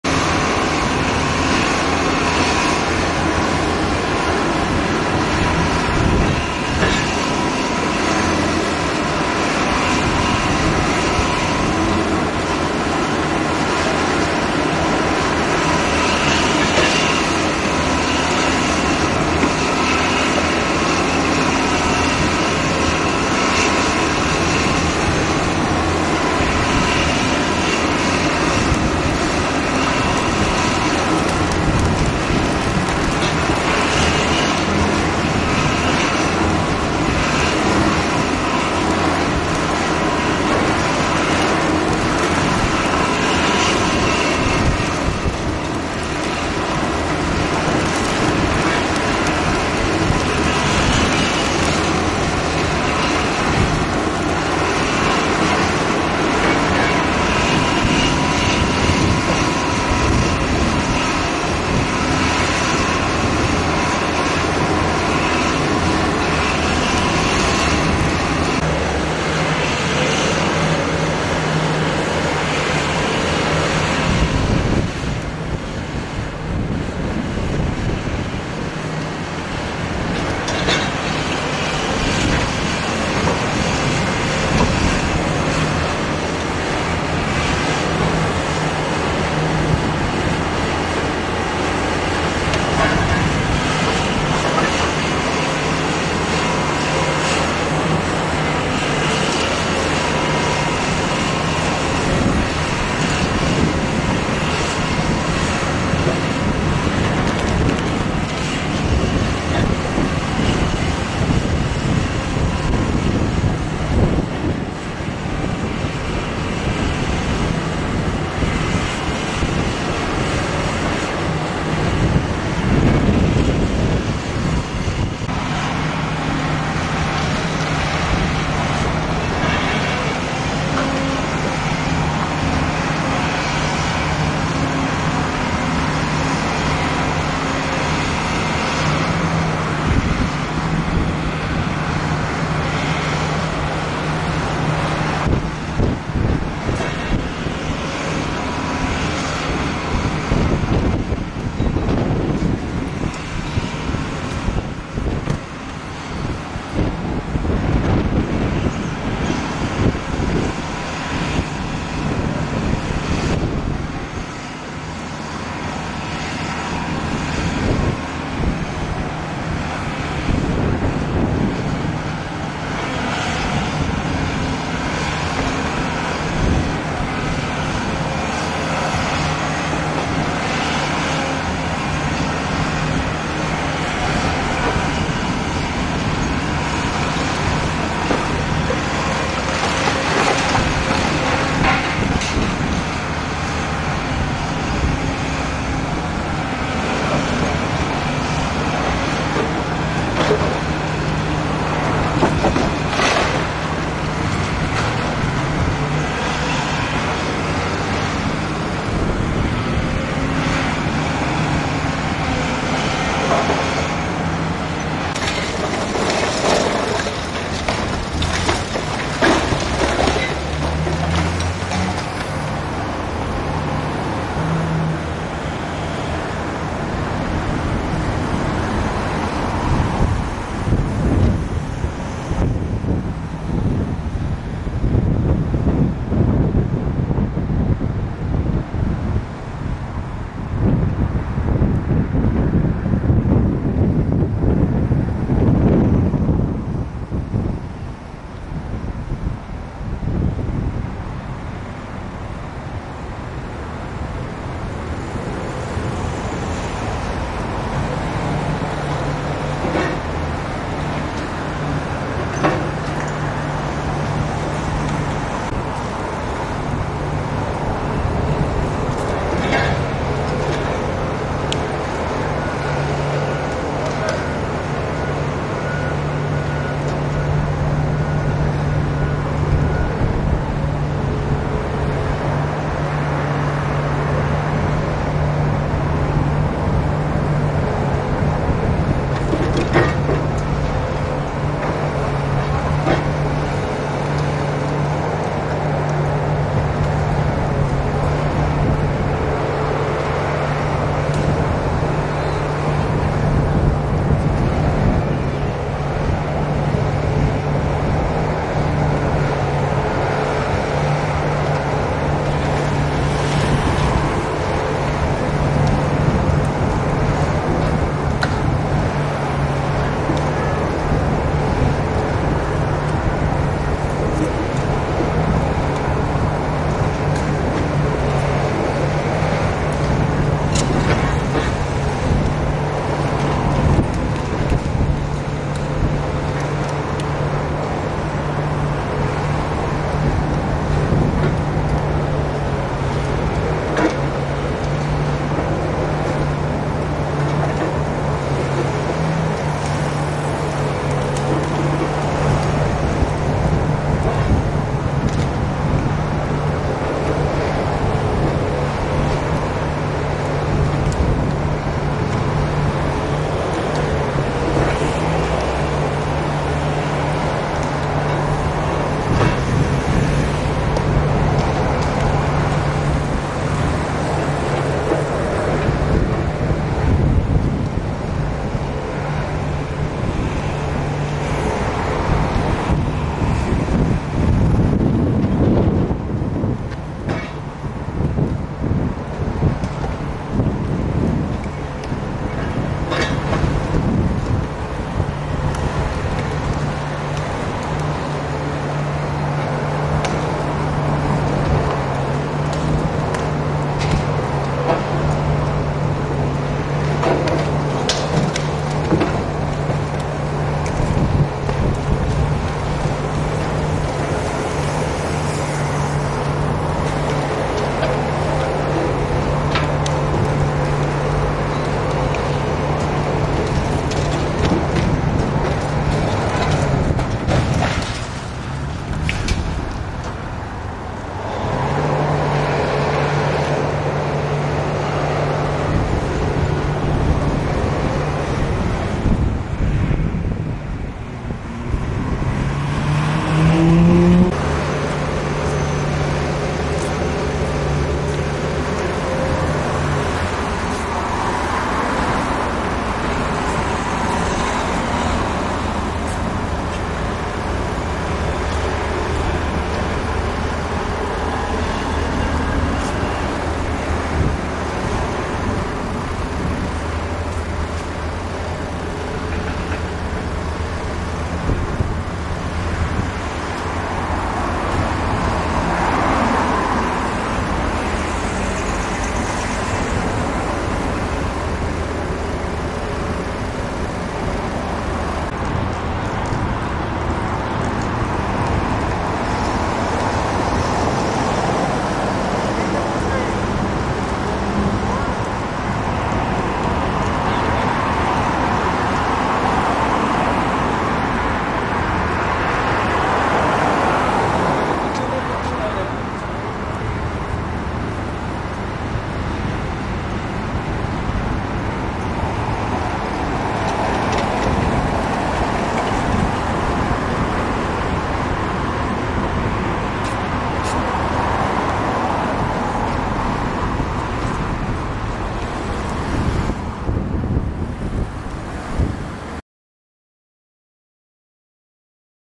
Tower Block Larne Demo
The demolition of one of the tower blocks in Larne Co Antrim back in I think it was 2011. I managed to shoot this video one evening after work unfortunately it was the middle of winter and the days were short...